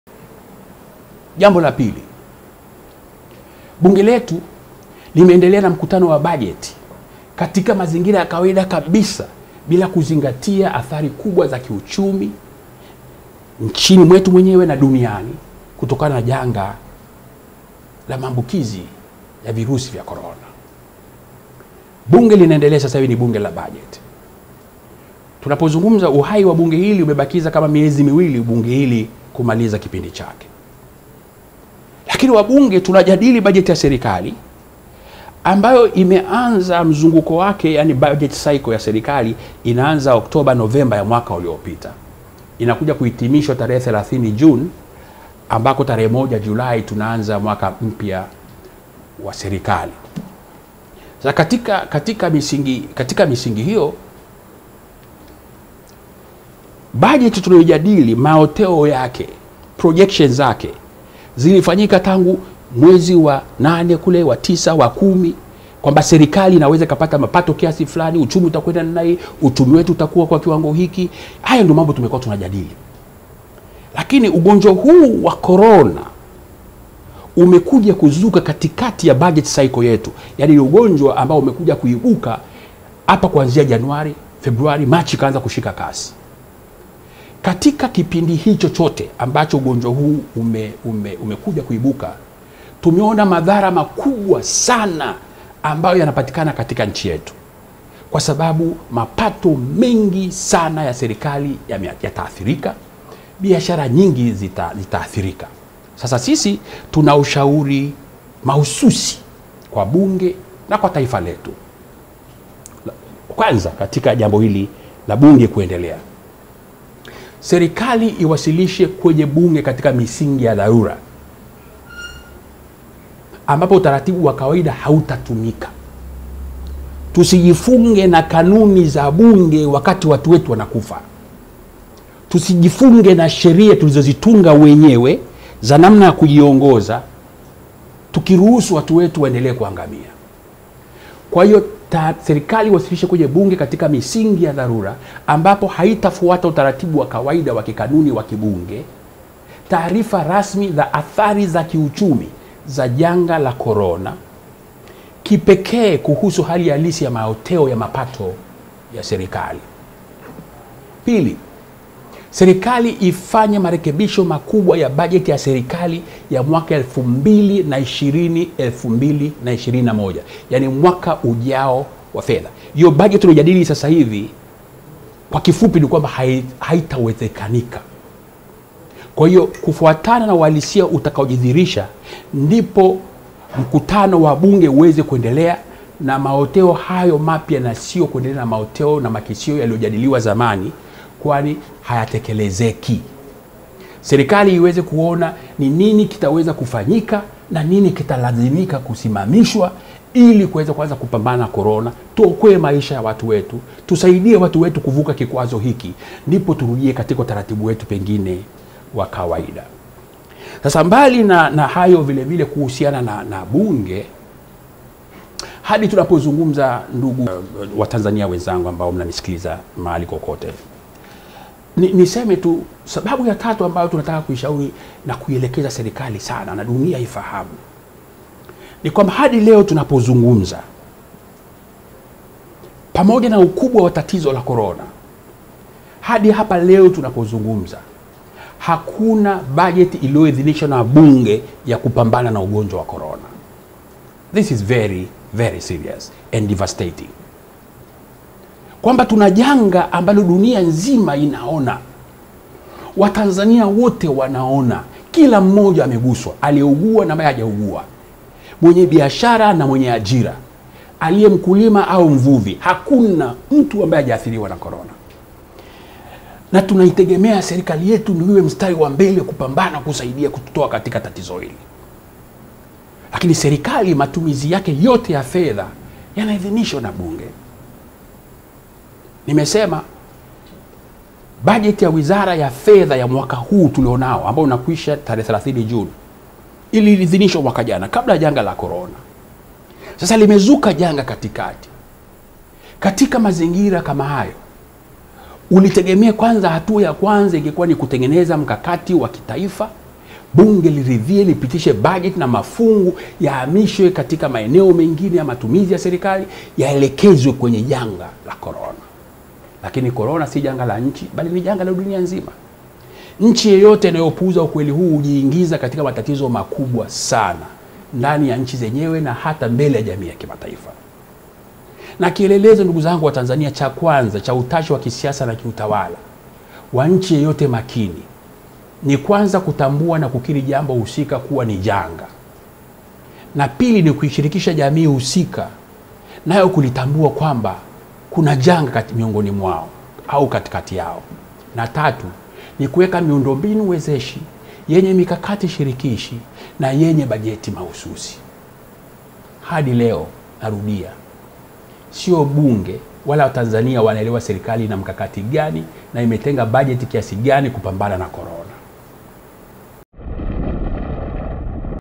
Bunge lashauriwa kuandaa bajeti maalumu ya kupambana na Corona
Mbowe asema bajeti ilipangwa imeshavurugwa, serikali na Bunge vifanye uamuzi mgumu wa kupanga bajeti ya dharura